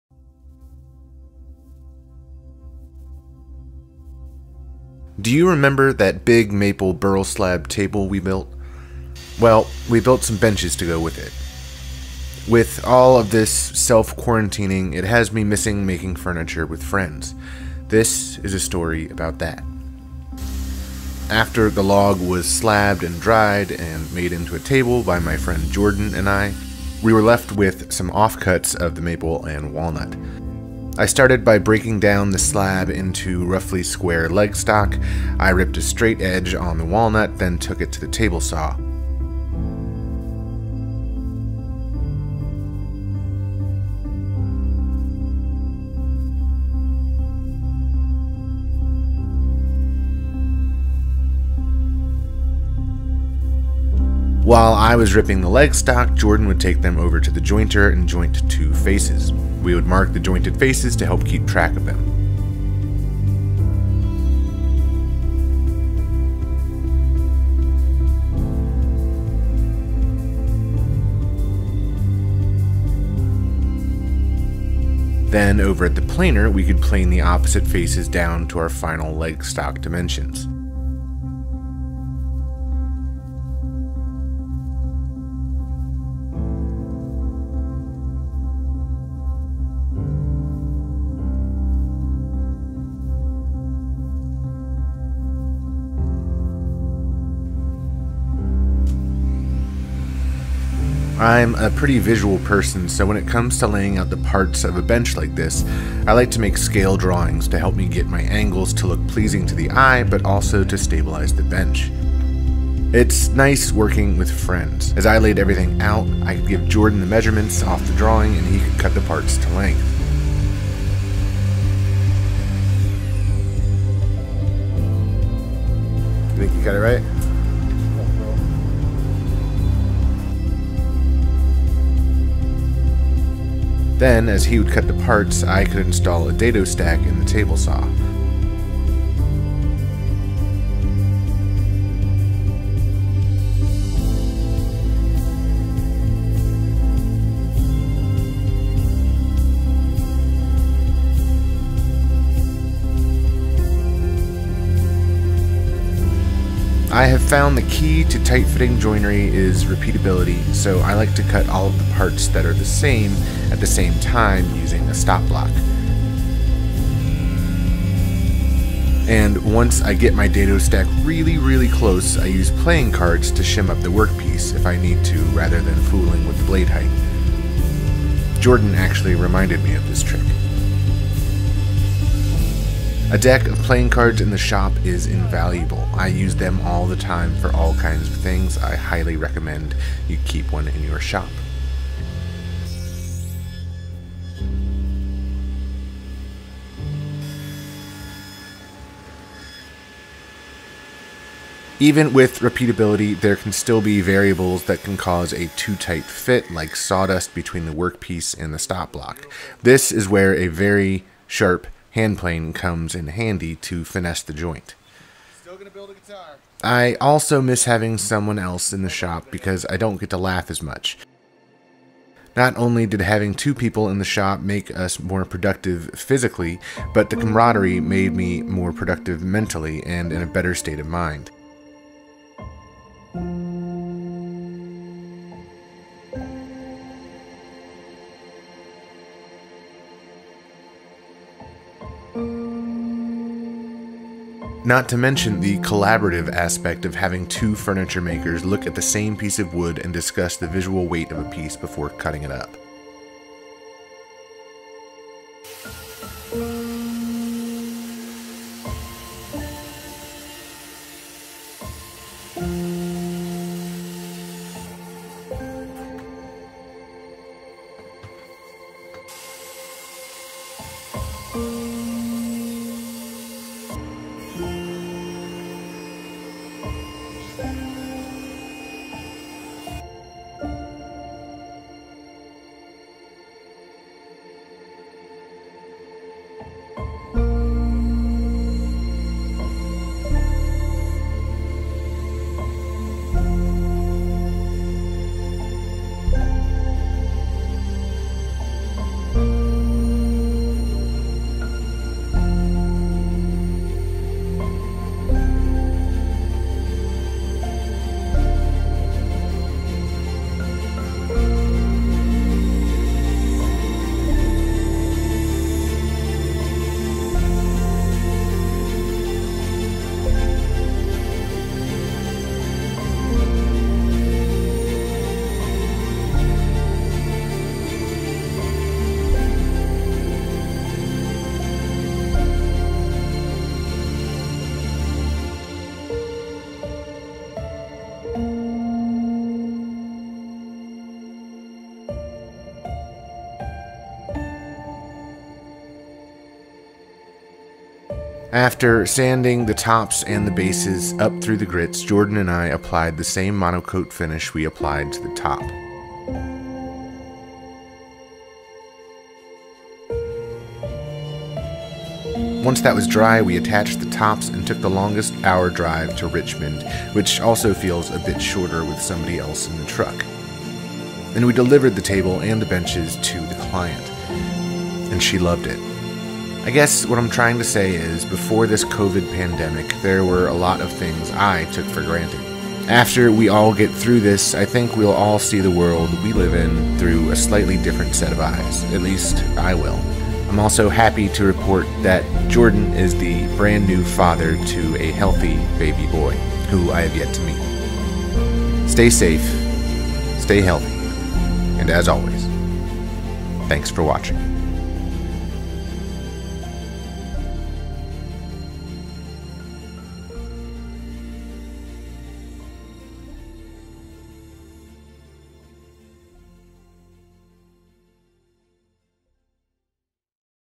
Making Benches for the Nakashima Table I Miss My Friends
A story of how my friend Jordan and I made two custom benches for seating around the Nakashima inspired Conoid Maple Burl Slab Table. I've made live-edge benches in the past and this is the Wm. Walker Co. design.